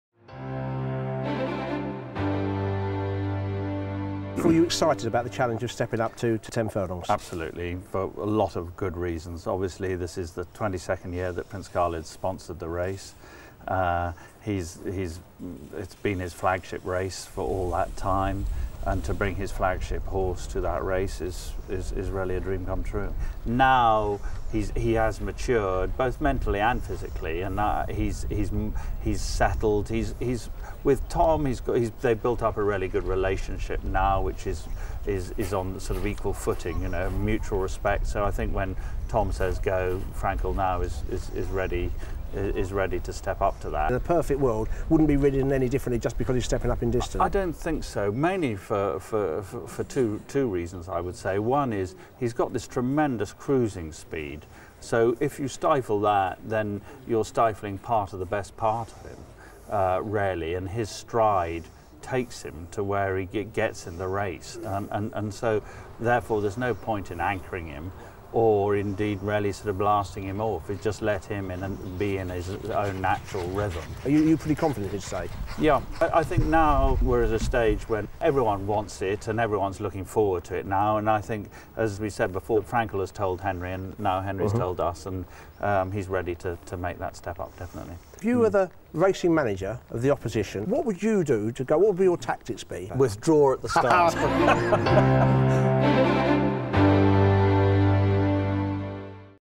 Lord Grimthorpe interview
Lord Grimthorpe, racing manager to Prince Khalid Abdulla, owner of unbeaten superstar Frankel, talks about the colt stepping up in distance for the first time in his next race, the Juddmonte International Stakes at York.